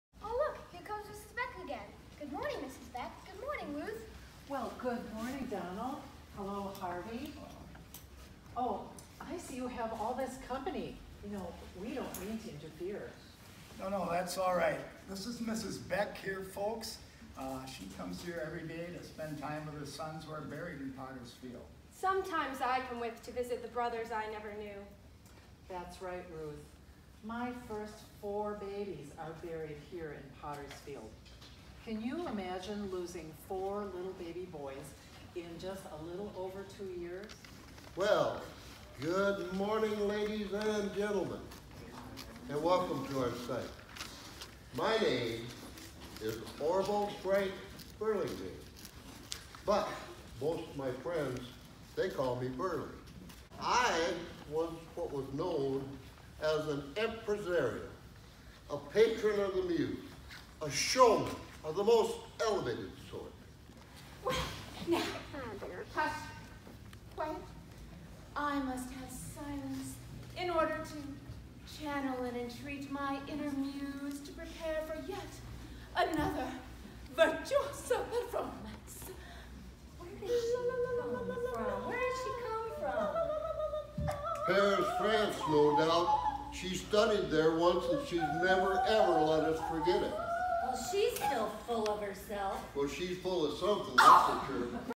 Winona's Voices of the Past Cemetery Walk
On Wednesday, Voices of the Past actors portrayed the characters of Winona's past to a group of school kids at the Winona County History Center.
The event will be open to the public at 7 p.m. Thursday at the History Center and from noon to 3 p.m. Saturday and Sunday at Woodlawn Cemetery.